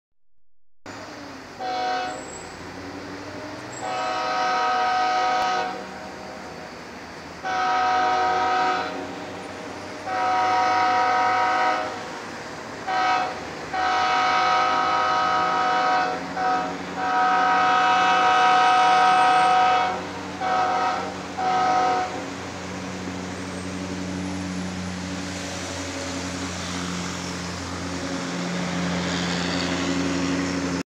KCSM#4903 LOCAL XALAPA/VERACRUZ LLEGANDO A CIRCUITO PRESIDENTES.
Un tren llegando a la altura de circuito presidentes vista desde un puente donde se 0uede apreciar un lindo paisaje...